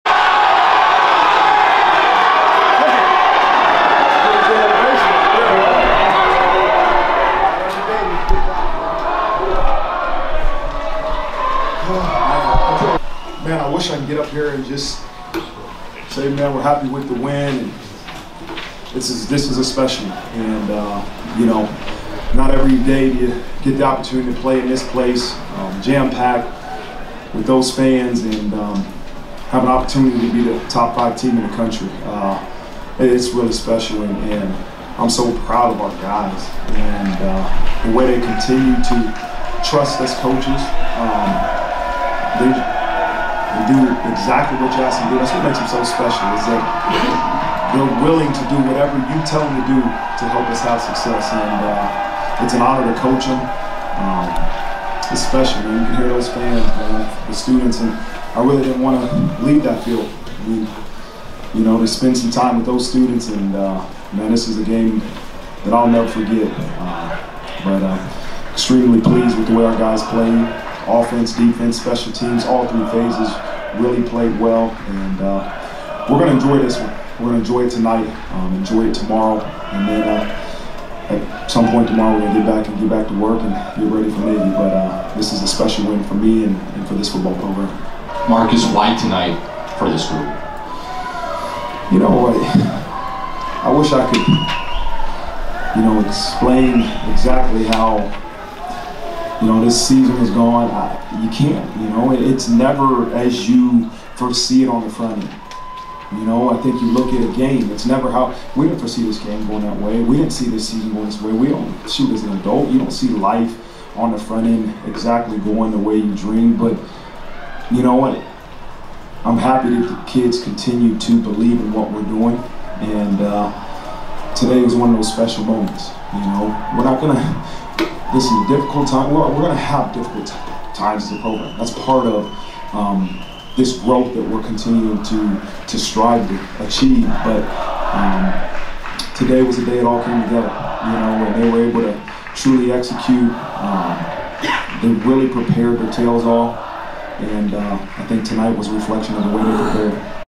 Notre Dame HC Marcus Freeman Talks Win Over No. 4 Clemson
Notre Dame football coach Marcus Freeman spoke following the 35-14 win over No. 4 Clemson on Saturday evening.

0:00 - On the win

1:28 - Why tonight was the night for everything to click